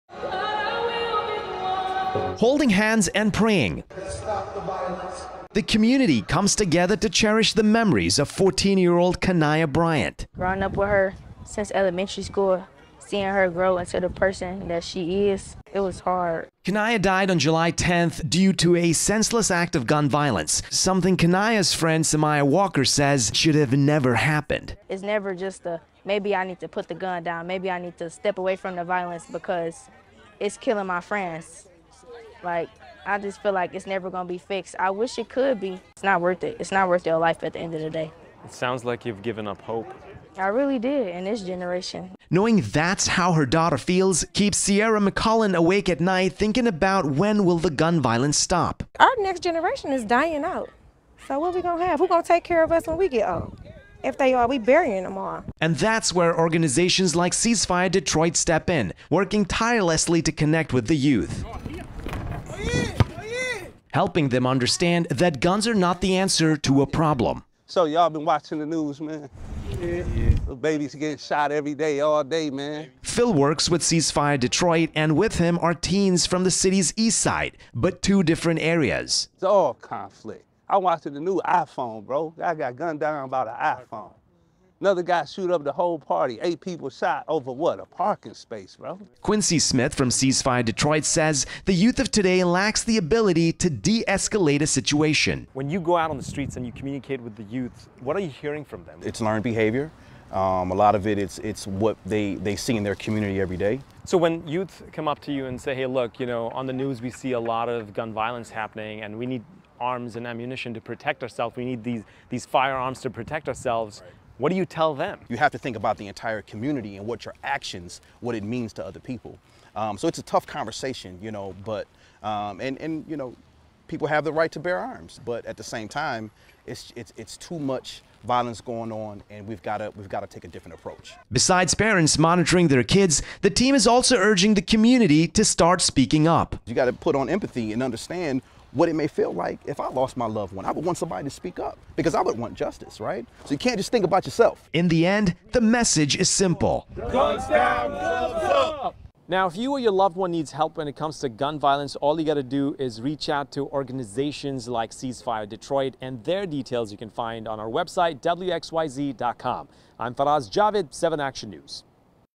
Gun violence tearing communities apart as organizations work to help victims
Gun violence is tearing communities apart as organizations work to help victims. Gun violence continues to impact the youth across Detroit. Detroit Police Department’s crime statistics show in 2021 a total of 77 individuals under the age of 18 were victims of gun violence, seven died.